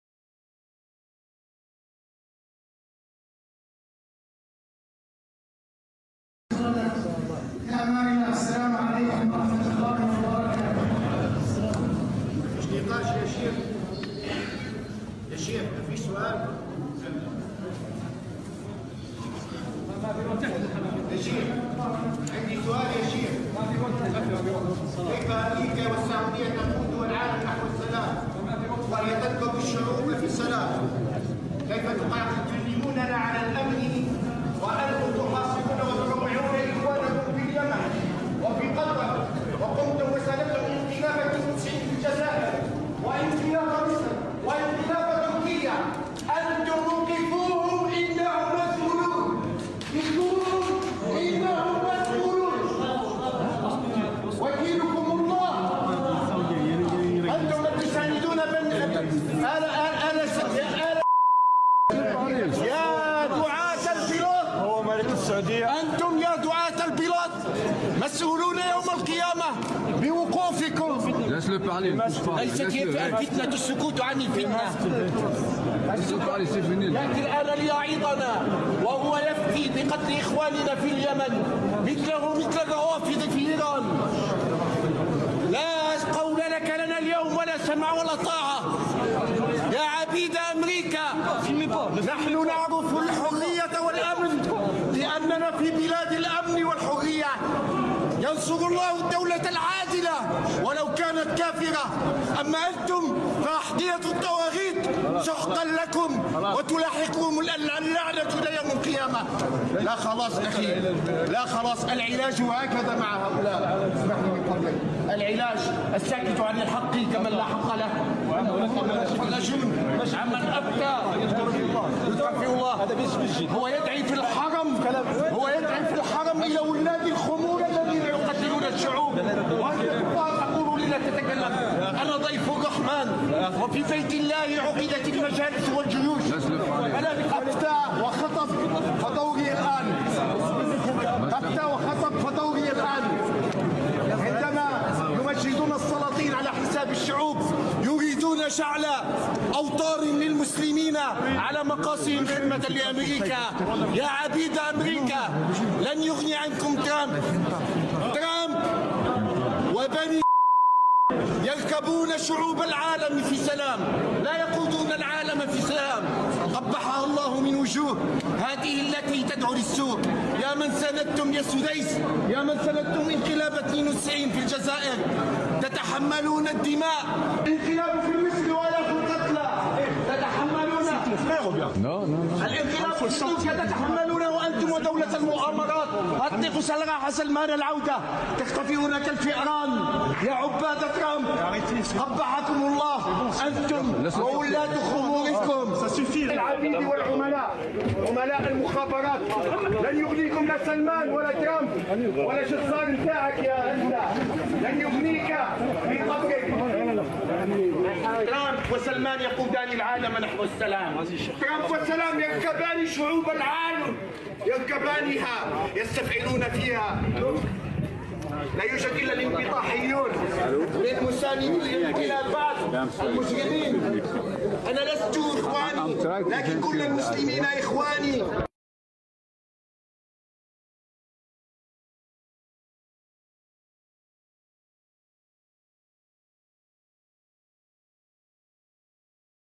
Muslim Activist exposes the hypocrisy of Sudais
English Subtitles: The Imam of the Grand Mosque in Makkah, Shaykh as- Sudais, is publicly accounted by a member of the congregation in Geneva Mosque for praising America as a "peacemaker" and backing Saudi Arabia's war in Yemen and boycott of Qatar.